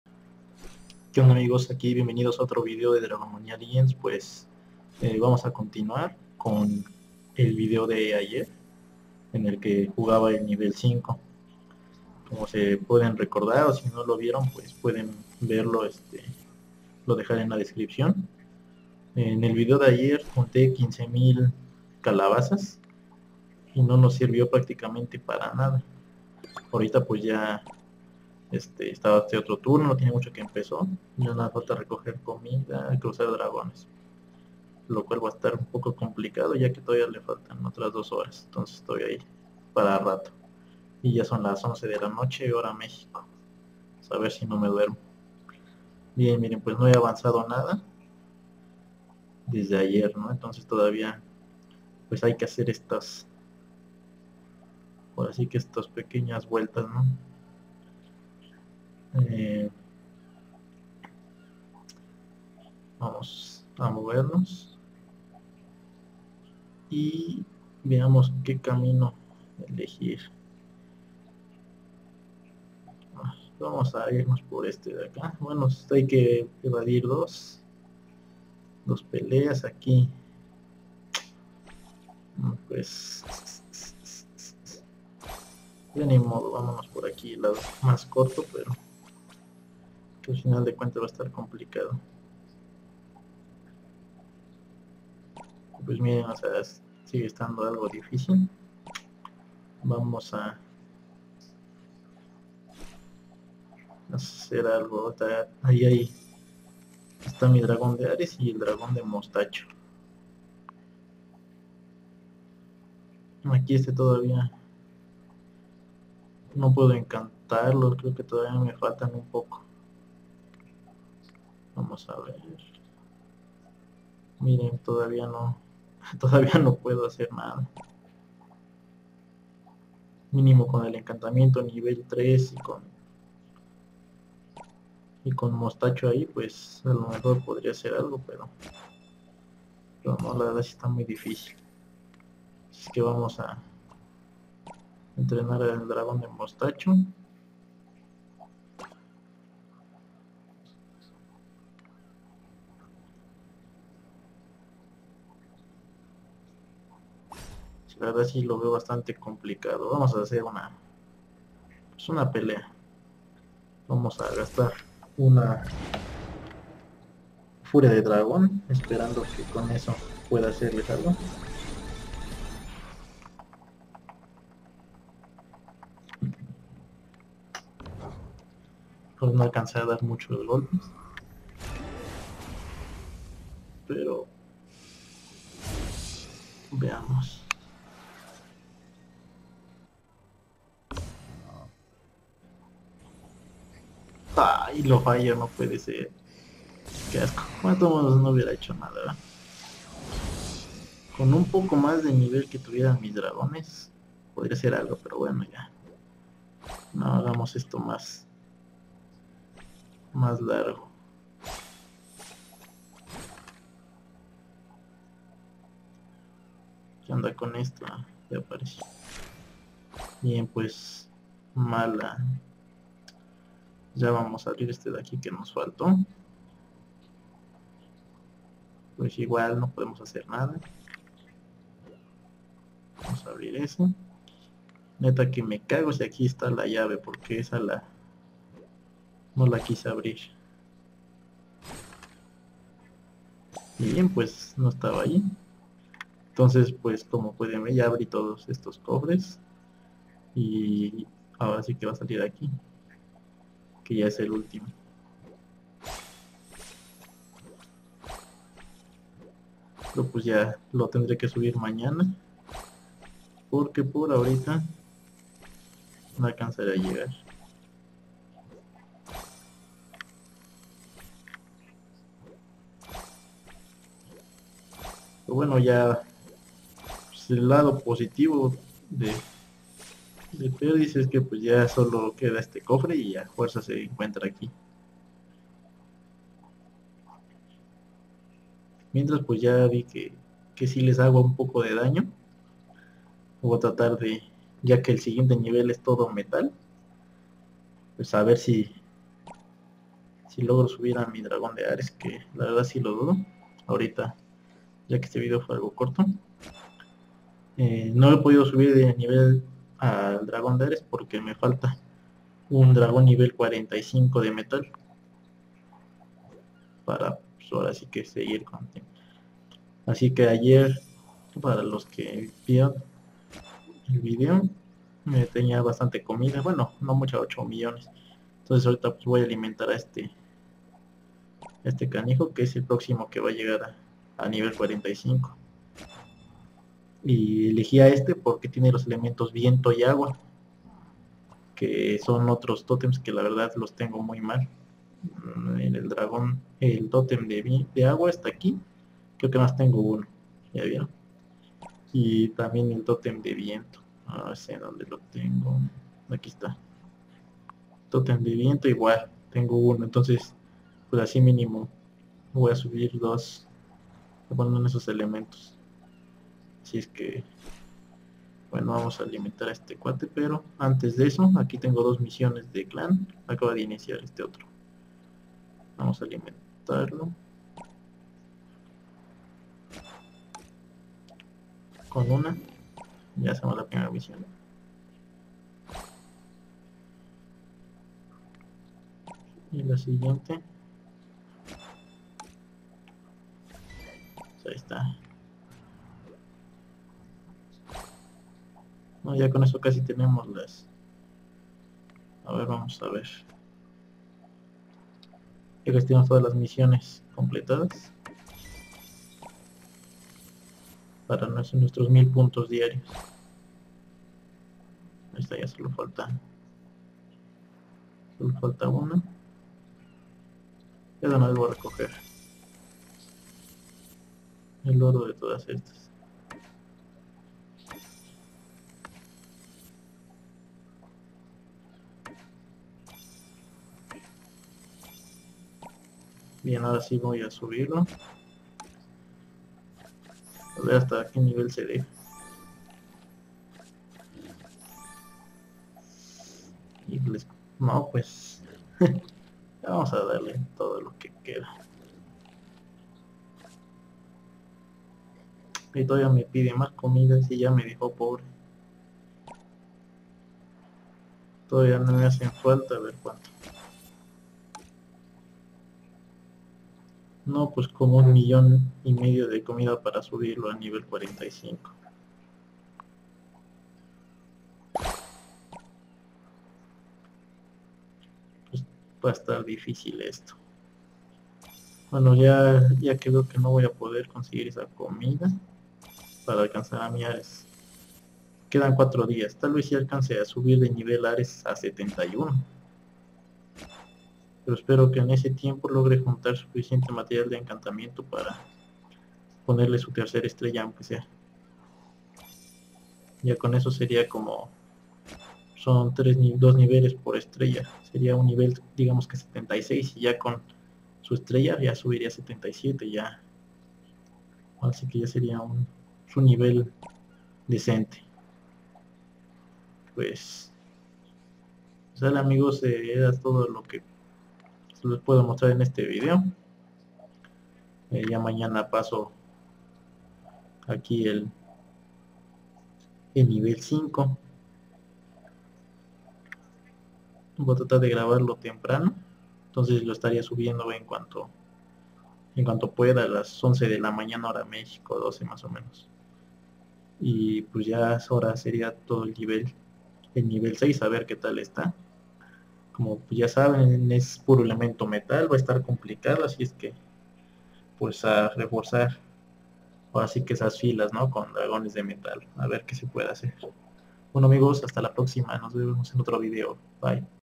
Evento La búsqueda del disfraz mala suerte en el nivel 5 T_T
Hola amigos bueno en este video les continuo el nivel 5 pero con mala suerte como podrán ver en el video.